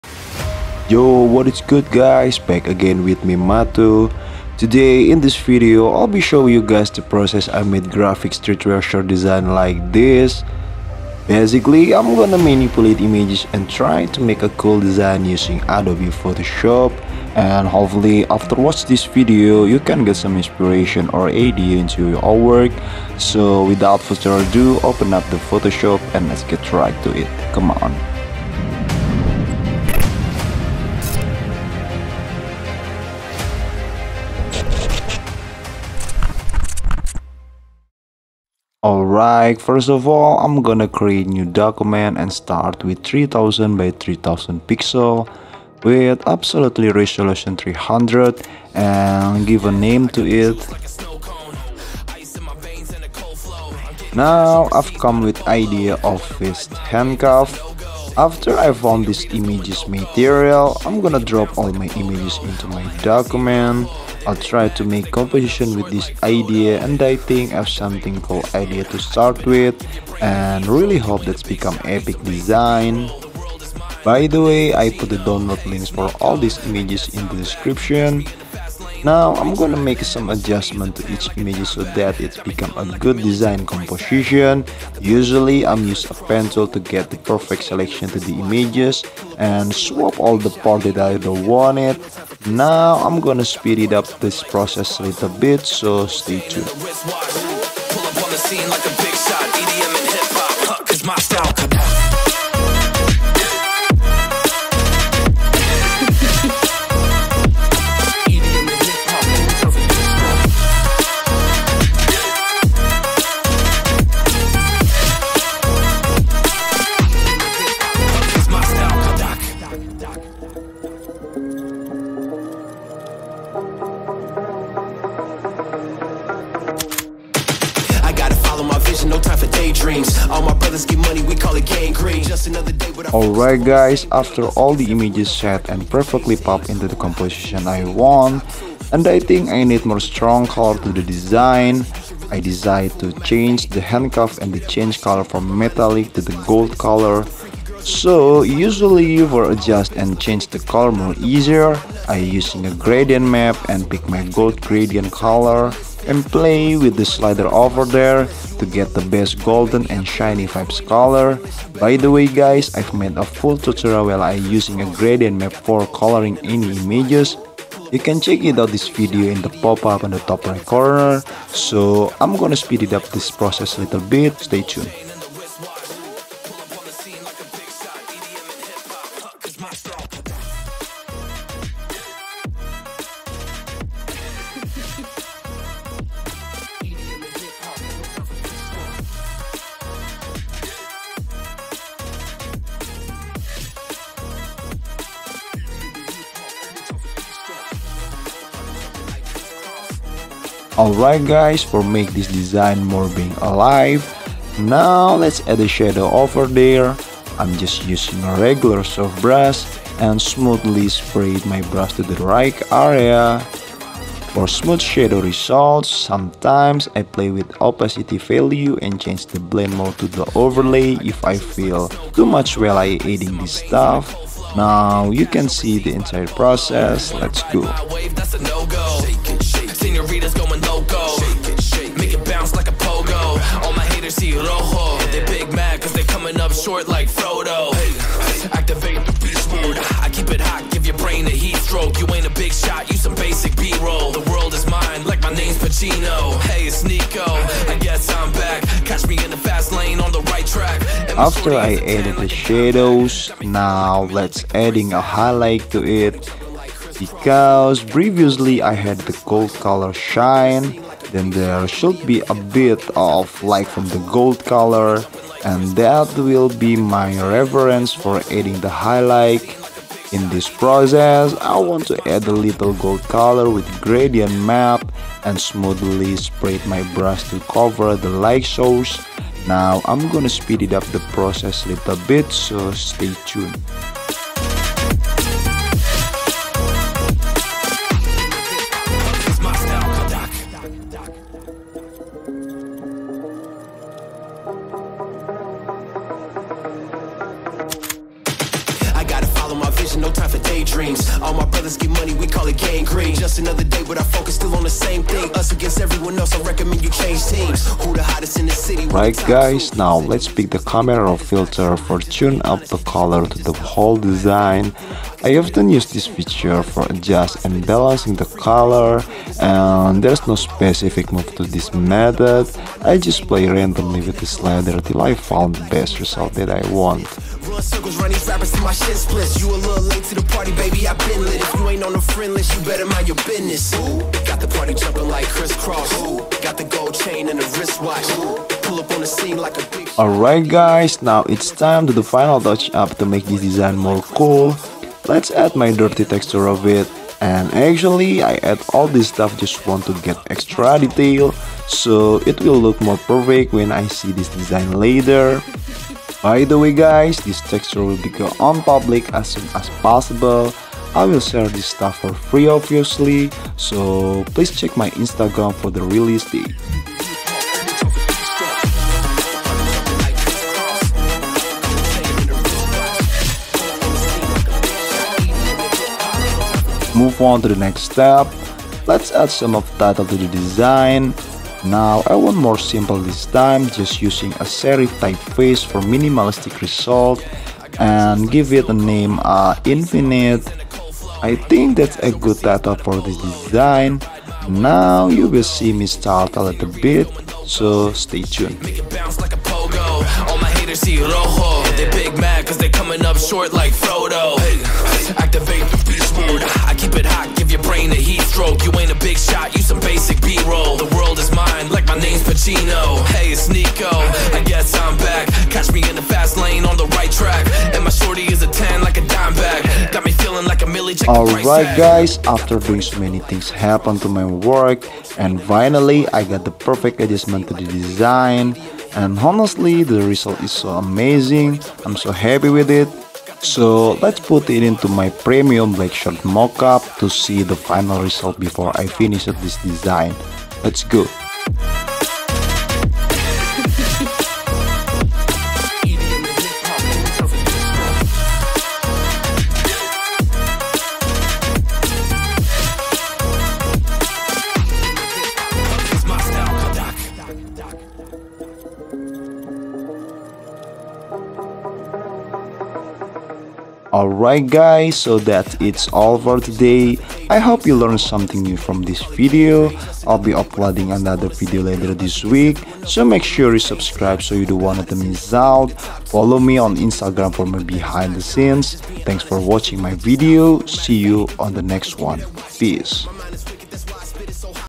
2023 STREETWEAR TSHIRT DESIGN (PHOTOSHOP)
In this video, I will designing an original Merch Design into realistic way using Adobe Photoshop! Combine several elements into one, resulting a unique designs for a tshirt.

its makes a difference. i really appreciate for your support guys.